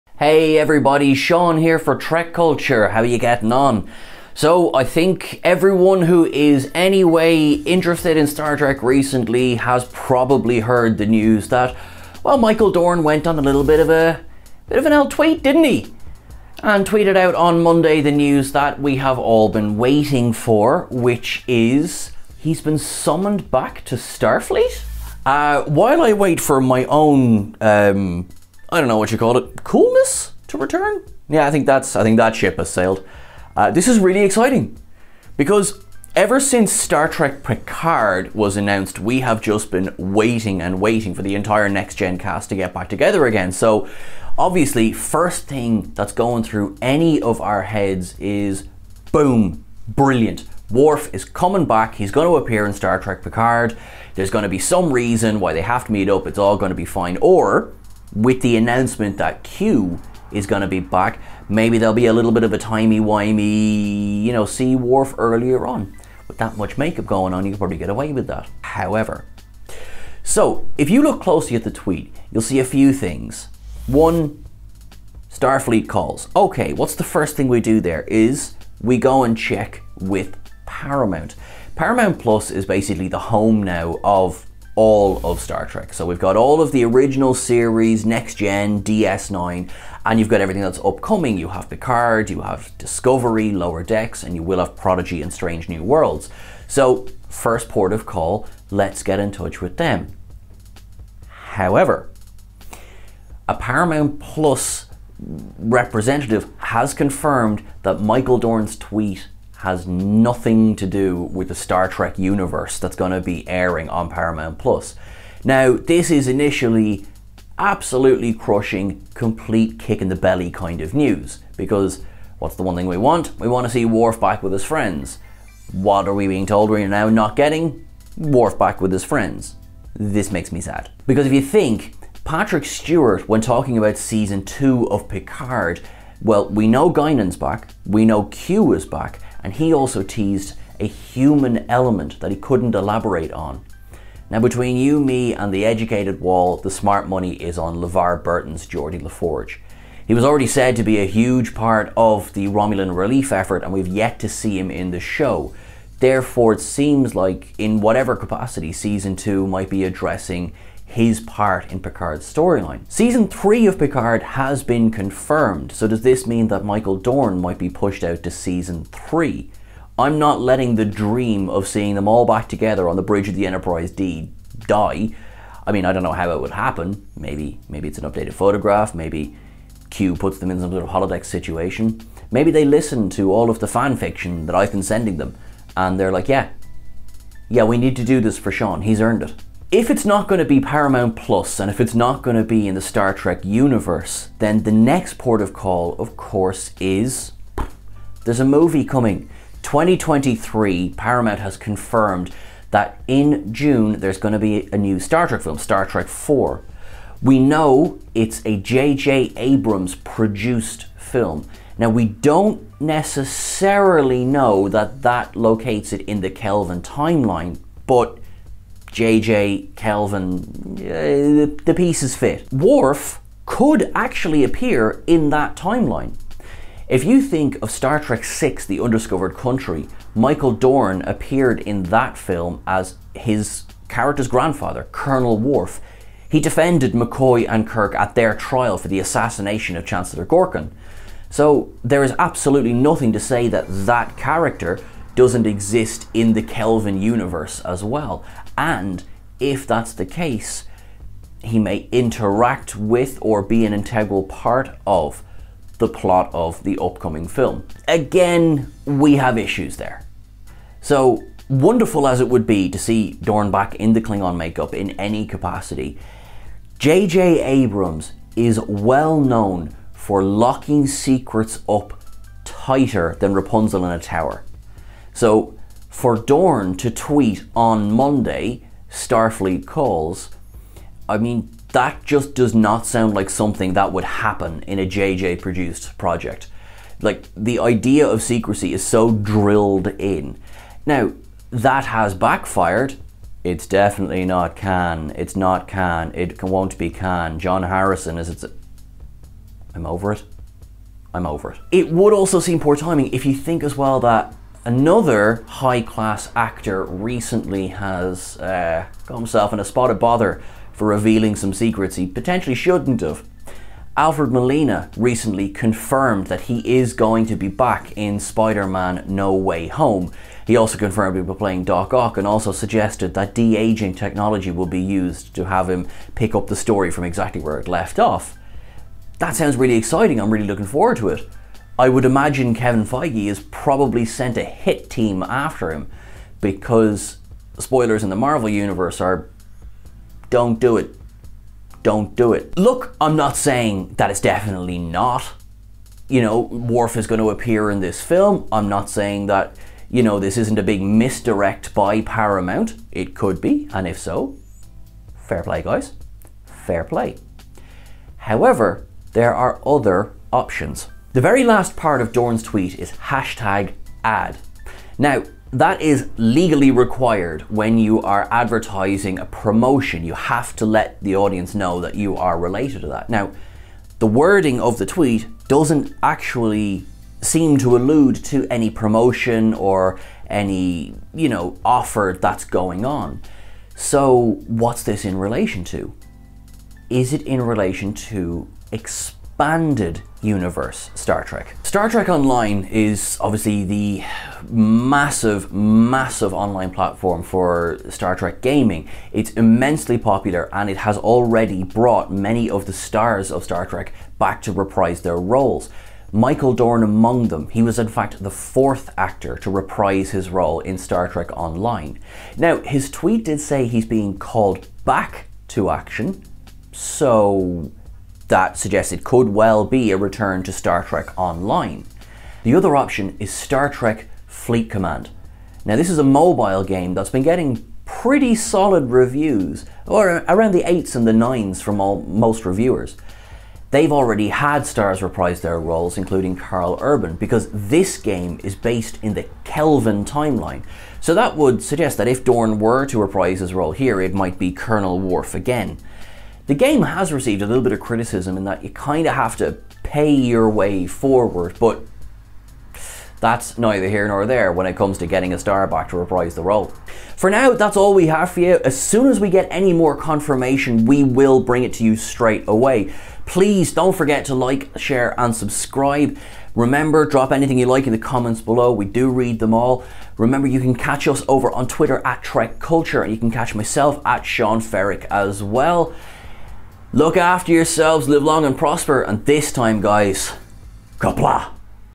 Michael Dorn Confirms Worf's Return To Star Trek!
Tweeting last night, fan favourite Michael Dorn confirmed he would be returning to Star Trek in some capacity.